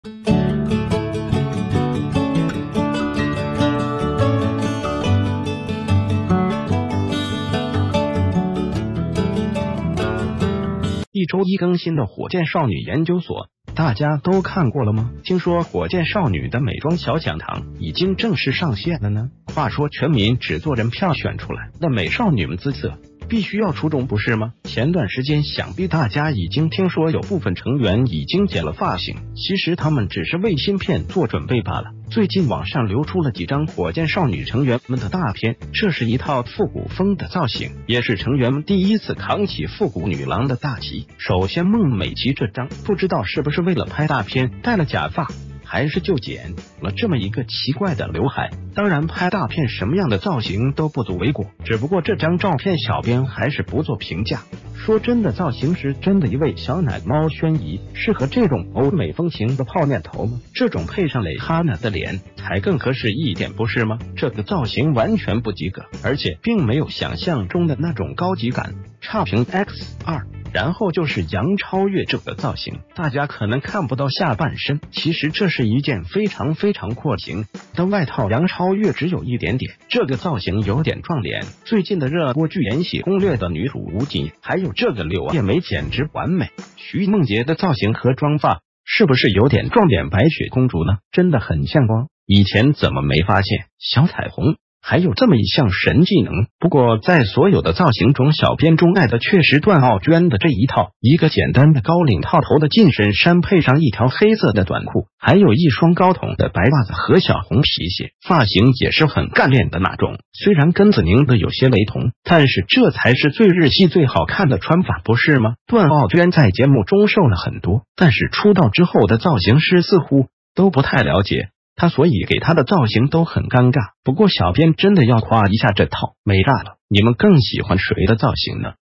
火箭少女最新雜志，宣儀的泡面頭一言難盡，最美的卻不是楊超越
一周一更新的《火箭少女研究所》大家都看過了嗎？聽說火箭少女的美妝小講堂已經正式上線了呢！話說全民只做人票選出來的美少女們姿色必須要出眾不是嗎？前段時間想必大家已經聽說有部分成員已經剪了發型，其實她們只是為新片做準備罷了。
    最近網上流出了幾張火箭少女成員們的大片，這是一套復古風的造型，也是成員們第一次扛起復古女郎的大旗，首先孟美岐這張，不知道是不是為了拍大片帶了假發還是就剪了這么一個奇怪的劉海，當然拍大片什么樣的造型都不足為過，只不過這張照片小編還是不做評價……
    說真的造型師真的一位小奶貓宣儀適合這種歐美風情的泡面頭嗎？這種配上蕾哈娜的臉才更合適一點不是嗎，這個造型完全不及格，而且并沒有想象中的那種高級感……差評X2；然后就是楊超越這個造型，大家可能看不到下半身，其實這是一件非常非常廓形的外套楊超越只有一點點，這個造型有點撞臉最近的熱播劇《延禧攻略》的女主吳謹言，還有這個柳葉眉簡直完美……
    徐夢潔的造型和妝發是不是有點撞臉白雪公主呢？真的很像哦！以前怎么沒發現小彩虹還有這么一項神技能，不過在所有的造型種小編鐘愛的確實段奧娟的這一套，一個簡單的高領套頭的近身衫配上一條黑色的短褲還有一雙高筒的白襪子和小紅皮鞋，發型也是很干練的那種雖然跟紫寧的有些雷同，但是這才是最日系最好看的穿法不是嗎？段奧娟在節目中瘦了很多，但是出道之后的造型師似乎都不太了解她所以給她的造型都很尷尬，不過小編真的要夸一下這套，美炸了！你們更喜歡誰的造型呢？歡迎留言！